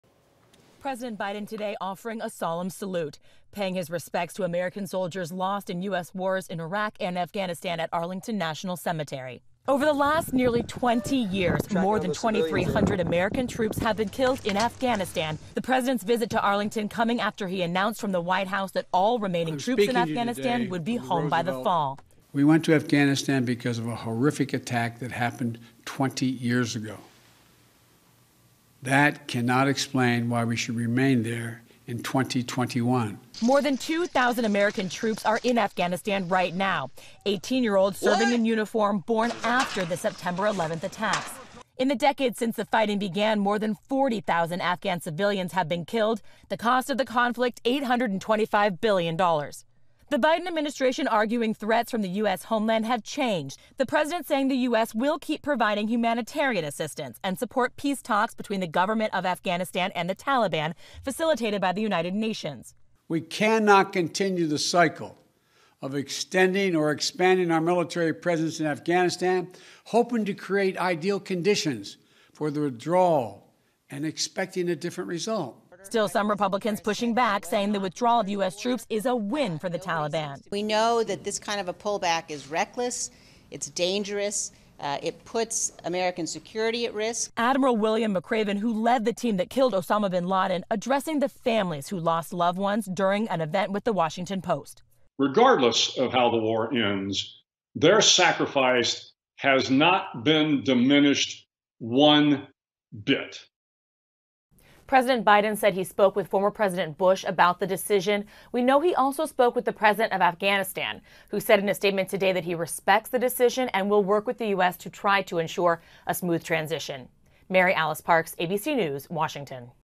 President Biden to pull US troops from Afghanistan, end 'forever war'
President Joe Biden said Wednesday he will withdraw remaining U.S. troops from the "forever war” in Afghanistan, declaring that the Sept. 11 terror attacks of 20 years ago cannot justify American forces still dying in the nation's longest war.

His plan is to pull out all American forces — numbering 2,500 now — by this Sept. 11, the anniversary of the attacks, which were coordinated from Afghanistan. Soon after Biden made his announcement, NATO chief Jens Stoltenberg in Brussels said the alliance had agreed to withdraw its roughly 7,000 forces from Afghanistan, matching Biden’s decision to begin a final pullout by May 1.

The U.S. cannot continue to pour resources into an intractable war and expect different results, Biden said.

The drawdown would begin rather than conclude by May 1, which has been the deadline for full withdrawal under a peace agreement the Trump administration reached with the Taliban last year.

“It is time to end America’s longest war,” Biden said, but he added that the U.S. will “not conduct a hasty rush to the exit."